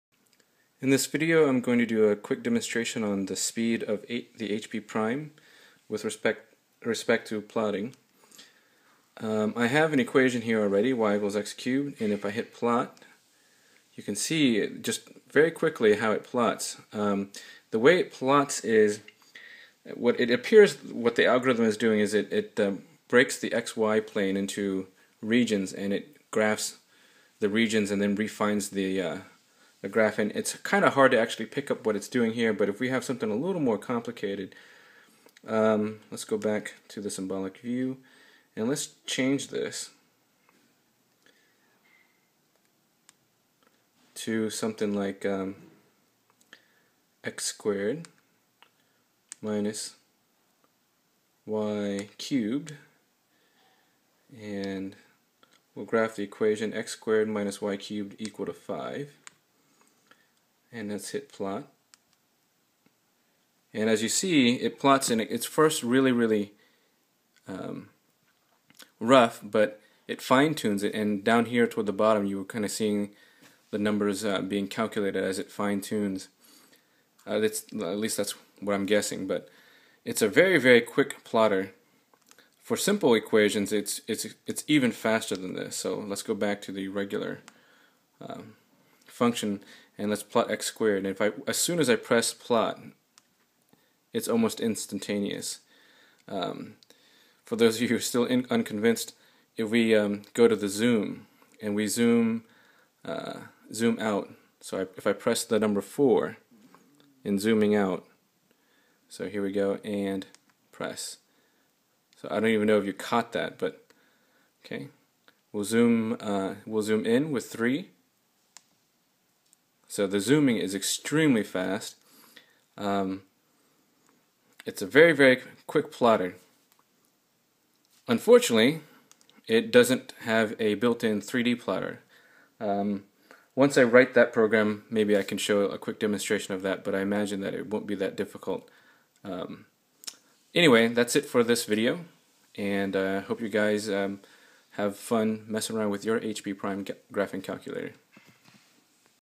HP Prime Graphing Speed
This is a quick video on how fast the HP Prime graphing calculator is able to create graph of functions and even equations in which the dependent variable is implicitly defined.